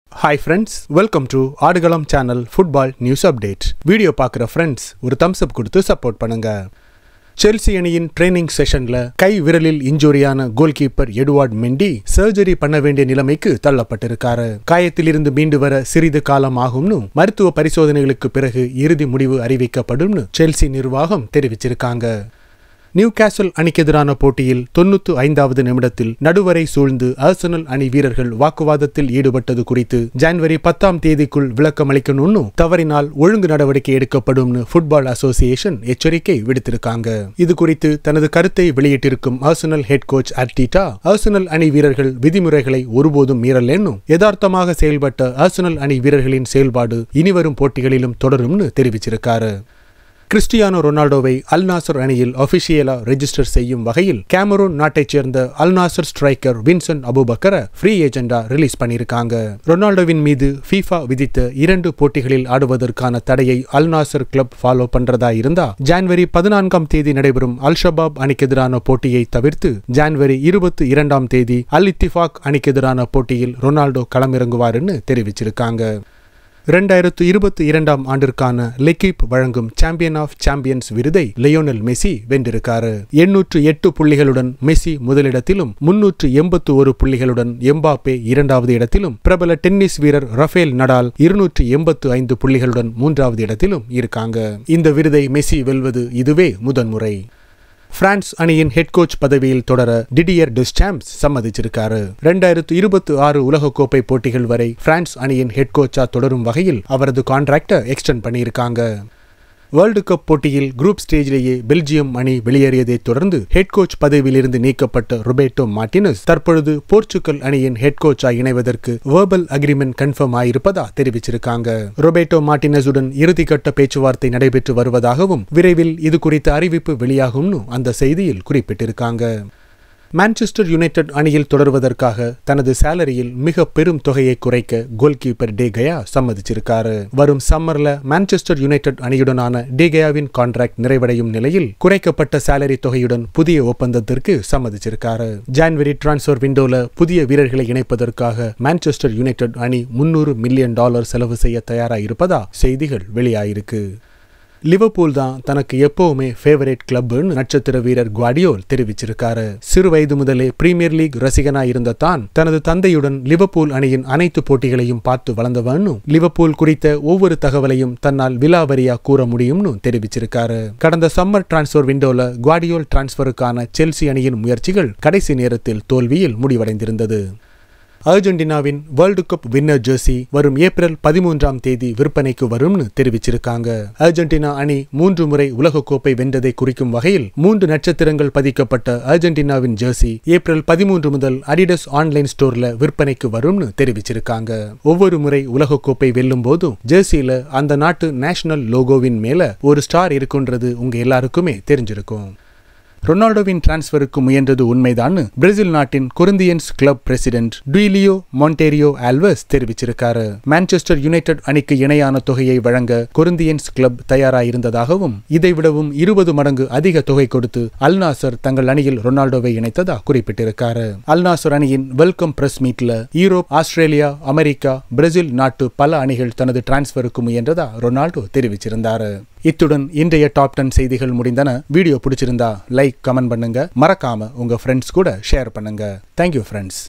கால்பந்து செய்திகள் 08th January 2023 | Football News Update | Tamil | Aadukalam
this is a daily update with the latest football news around the world. hope the fans like and enjoy the video. please mention your opinion in the comment box..